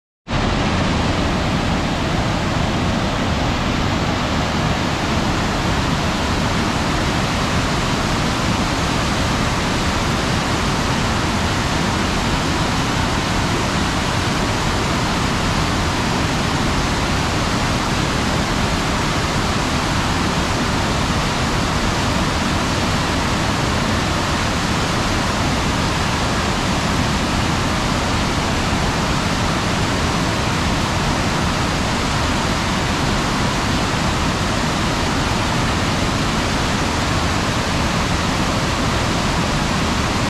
Natural Bridge, video 4, 2160p60
This is the natural bridge area of the Rogue River in Oregon.